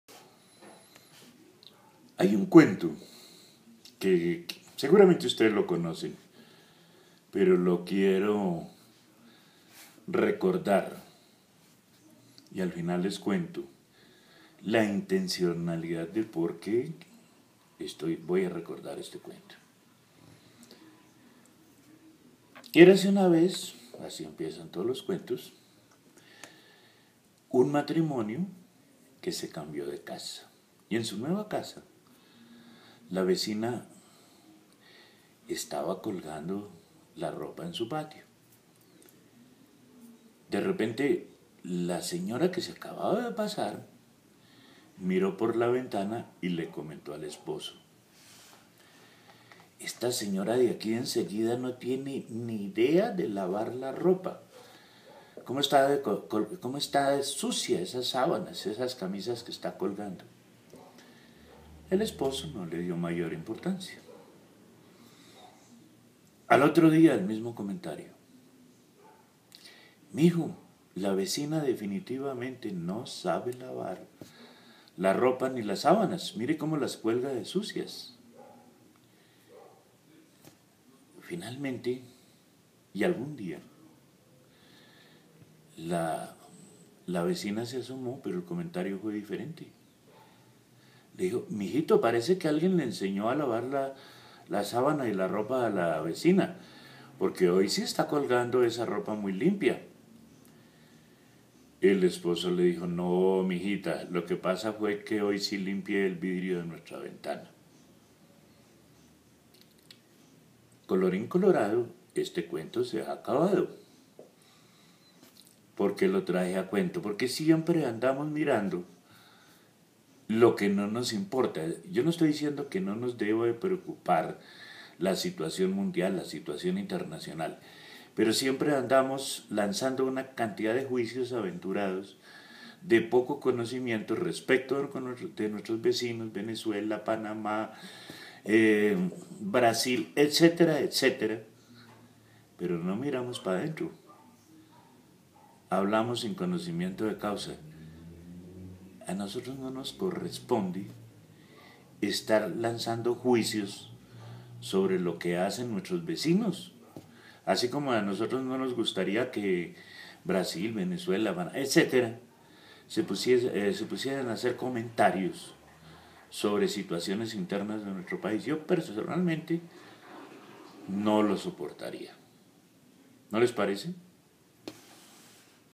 El cuento de la ventana y las sábanas sucias lo podemos aplicar en Colombia.
Andamos tan preocupados por los países vecinos que no vemos lo que pasa en casa.Eso es lo que desean los dueños de los medios de comunicación.